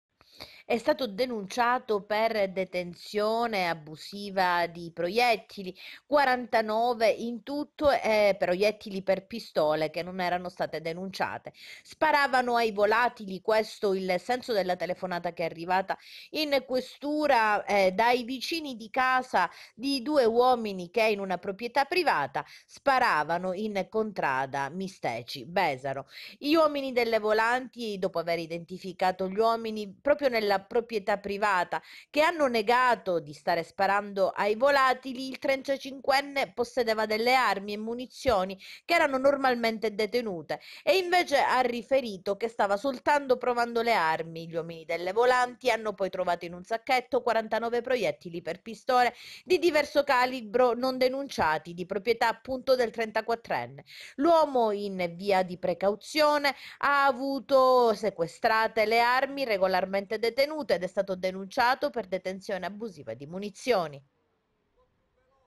Sparava ai volatili, denunciato
Misteci, denunciato dalle volanti trentaquattrenne per detenzione abusiva di munizioni, sequestrate 45 cartucce